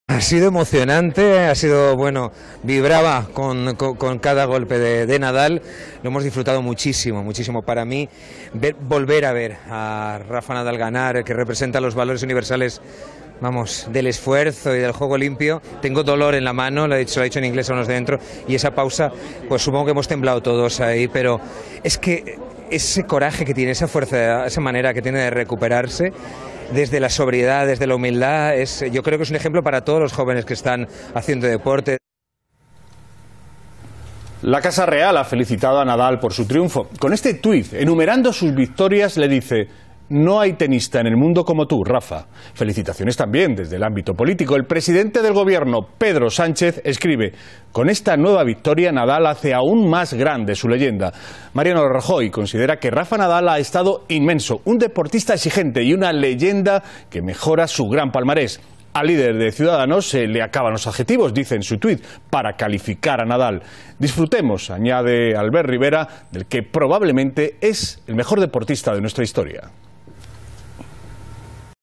Màxim Huerta felicita a Rafa Nadal por su 11er Roland Garros
Maxim Huerta: "Nadal es un ejemplo para todos los jóvenes deportistas"
10 jun 2018
El ministro de Cultura y Deporte ha reconocido a TVE estar emocionado por el undécimo Roland Garros de Rafa Nadal y ha puesto al tenista balear como ejemplo para todos los deportistas jóvenes. Huerta ha estado presente en la final en París.